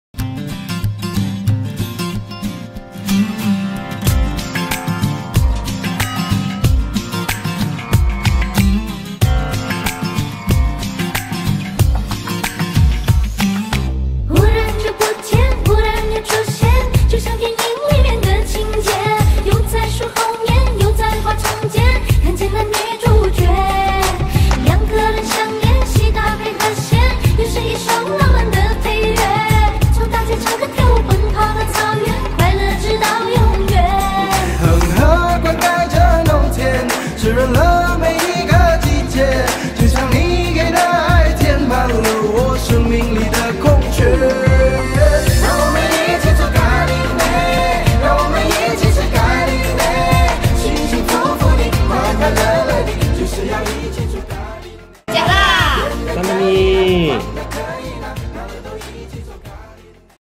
//槟城美食篇// 银行街最受欢迎的「阿里椰浆饭」！
在槟城里，最繁忙的土库街也称为"银行街"，每天都有不少的上班族与当地居民前往的一条街。在最多银行林立的这条街上有个小贩中心 - Sri Weld Food Court . 今天小编要介绍的是小贩中心门口的「阿里椰浆饭」, 马来文称为 Ali Nasi Lemak. 他们家的椰浆饭吸引了不少的人前来品尝！他们每天都会现场把新鲜的椰浆饭一包包的包起来。先把白饭摆好再放上酱与料 有鱼、虾、蛋、墨鱼 等 重点他们的价钱一点都不贵！

宣特工: JiakLah! 蛋哈密?!

阿里椰浆饭 Ali Nasi Lemak
地址：Sri Weld Food Court, Beach St, George Town, 10300 George Town, Pulau Pinang
营业时间：星期一至星期五 7:00 am - 4:00 pm
                   星期六 7:00 am - 2:00pm (星期日休息)

追踪我们Instagram : jiaklah_my